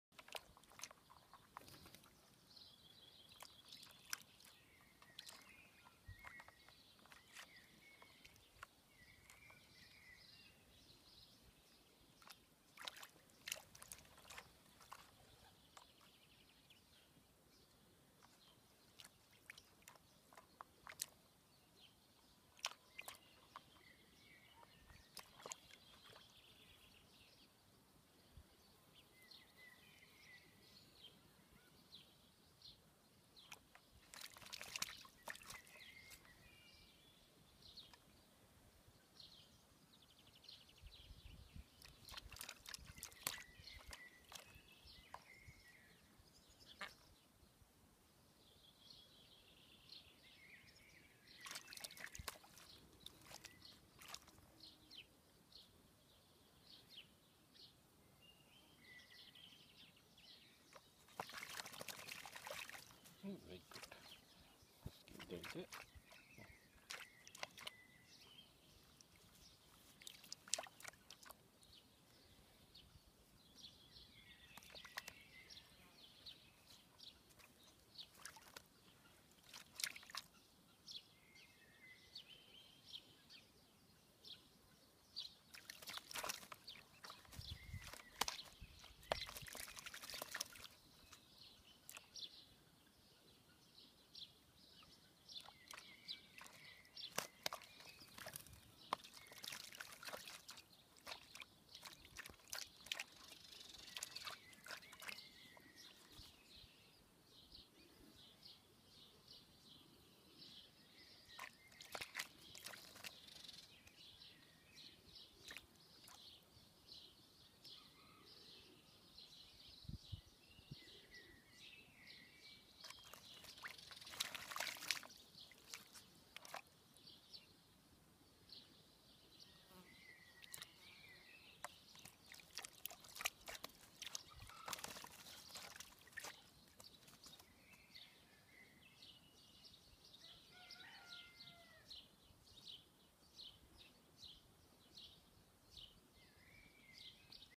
Baby Peregrine Falcon first bath at falconry centre Argyll Scotland
Young peregrine falcon having his first bath, and loving it.